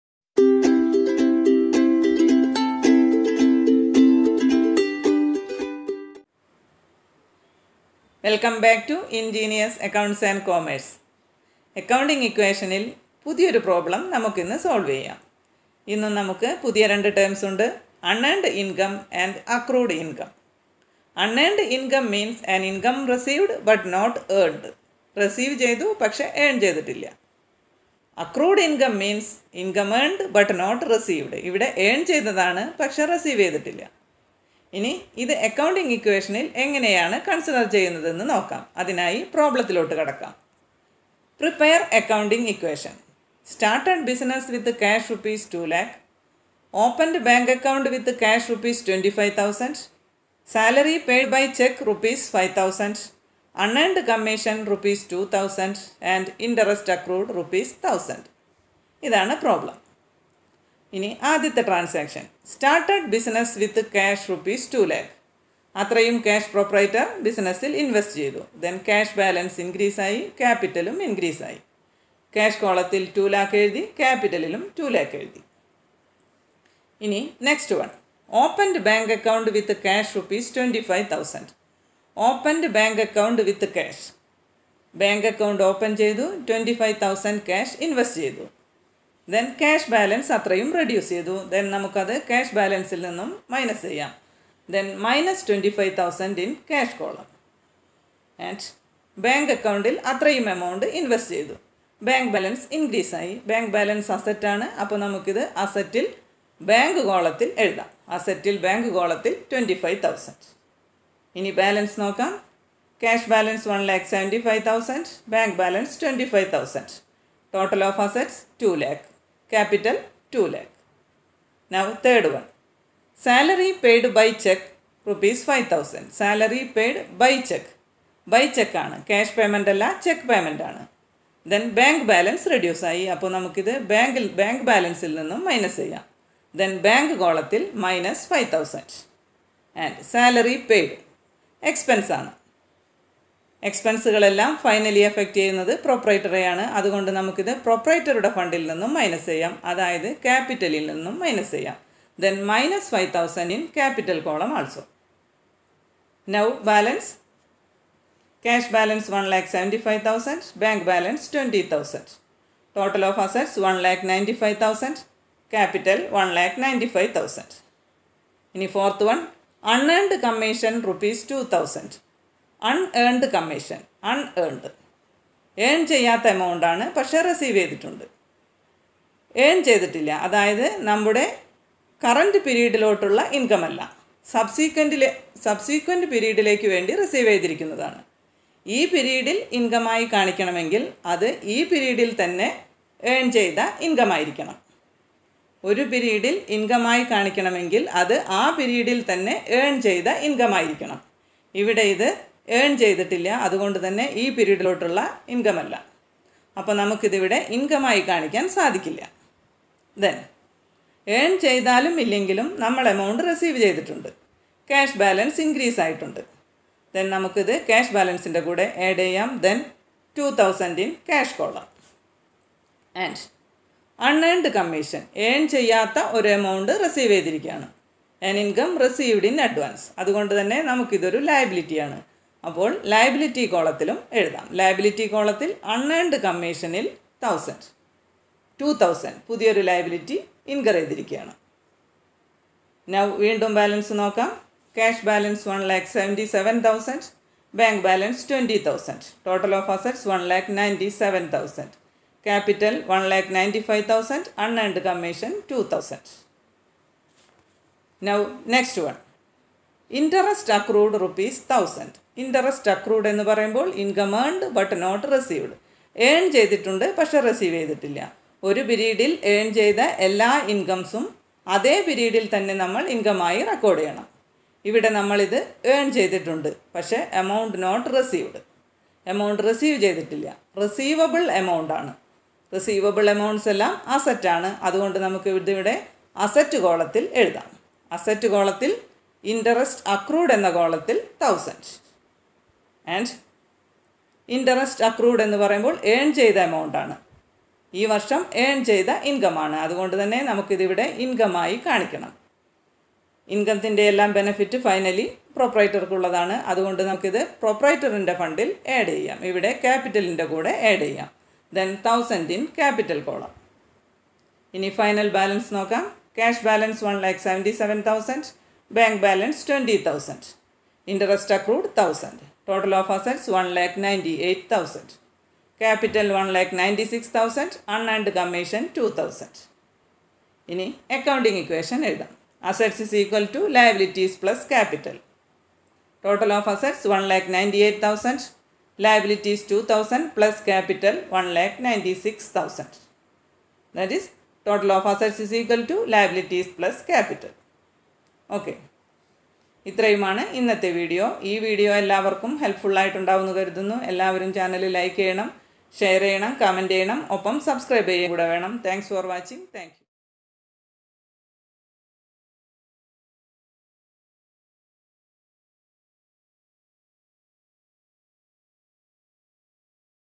Unearned income and accrued income in accounting equation(in Malayalam)
This video explains the treatment of unearned income and accrued income in accounting equation.Hope it would be helpful to all.